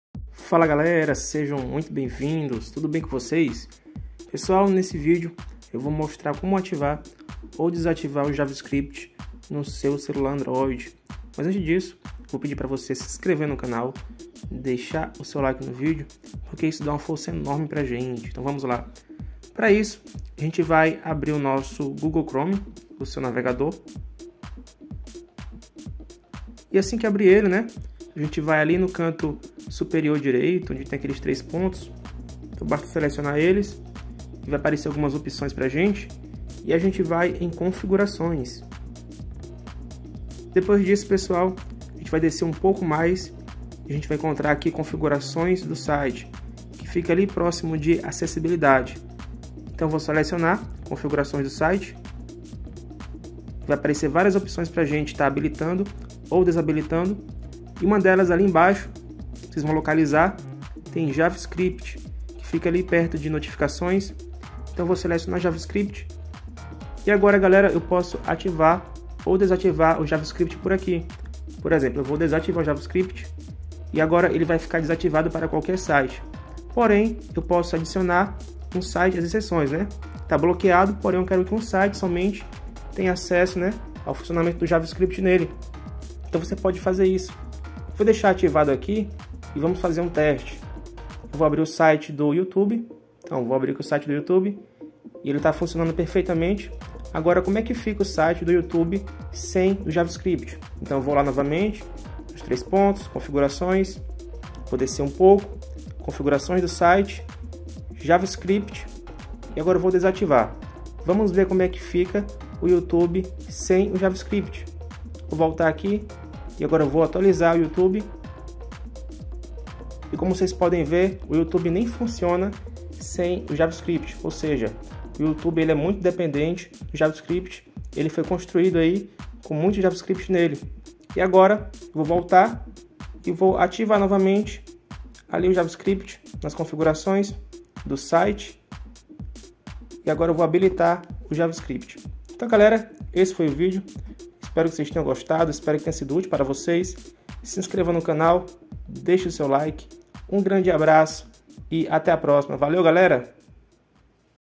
Como Ativar ou Desativar o JavaScript no Celular (Rápido e Fácil)
Nesse vídeo mostro como ativar ou desativar o JavaScript no seu celular Android de maneira simples e fácil. Sendo uma tecnologia muito importante para a internet nos dias de hoje, muitos programadores tem se dedicado a aprender JavaScript e entender seu funcionando. Pensando nisso foi preparado esse tutorial para auxiliar nesse aprendizado.